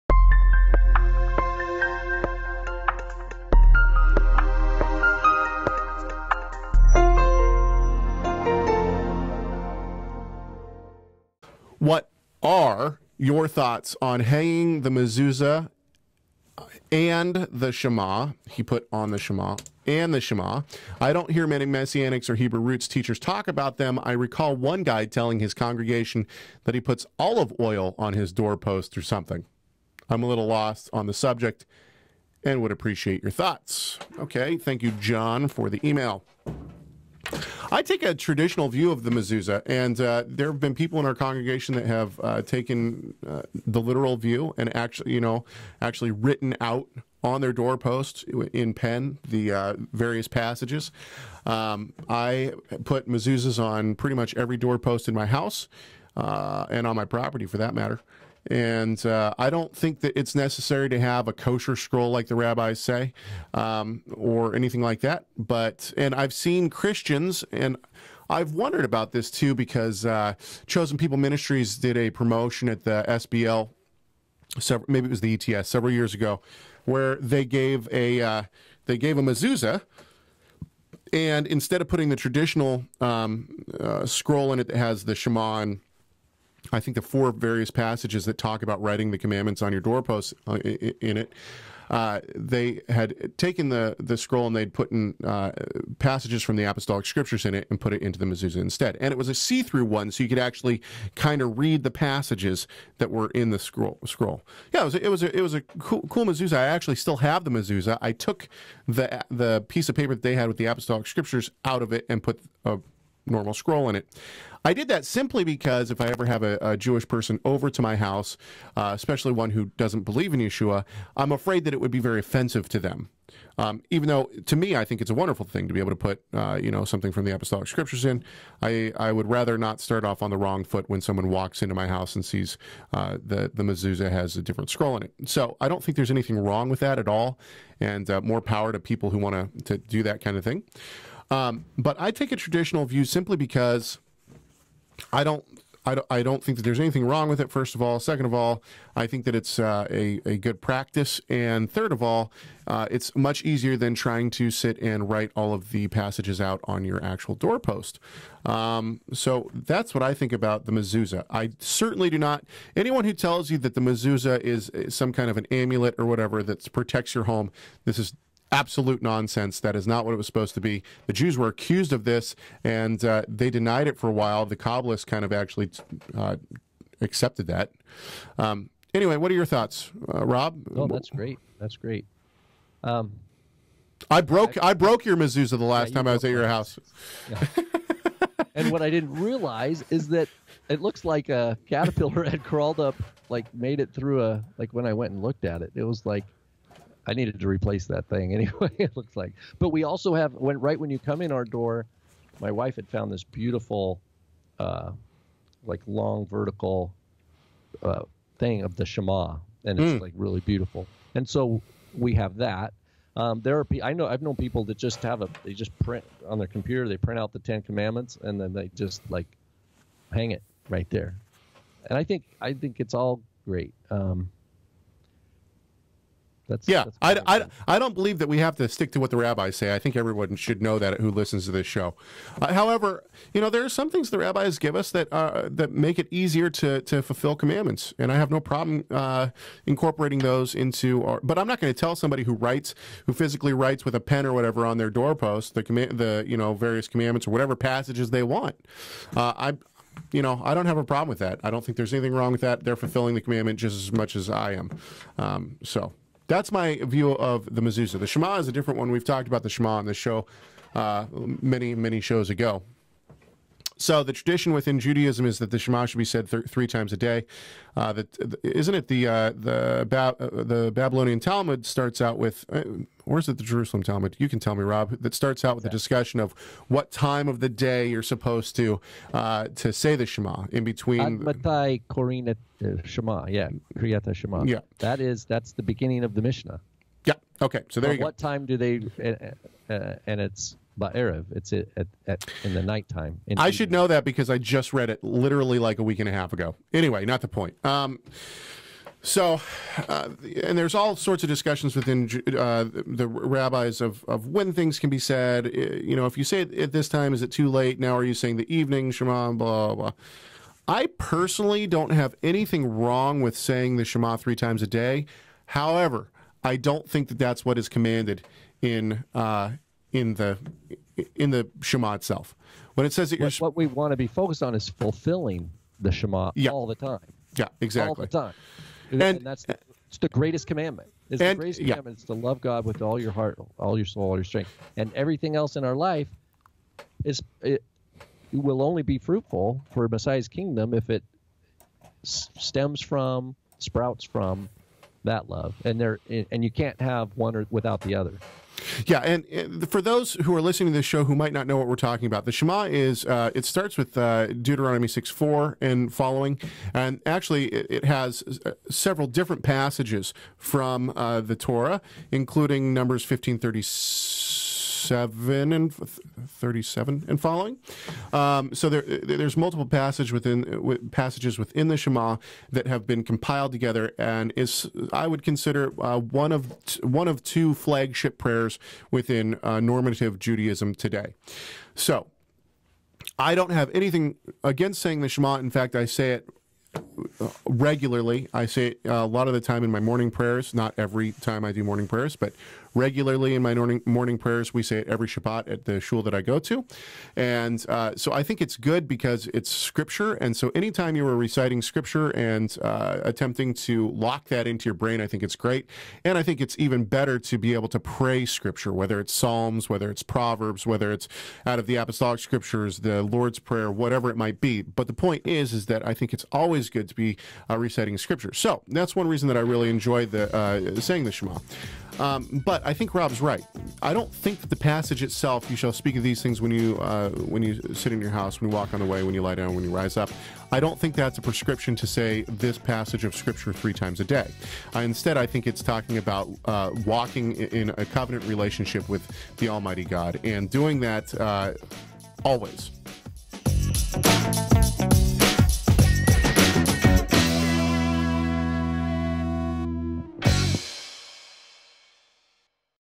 Messianics and Mezuza's - TorahResource
Messianics and Mezuza's and the Shema are discussed.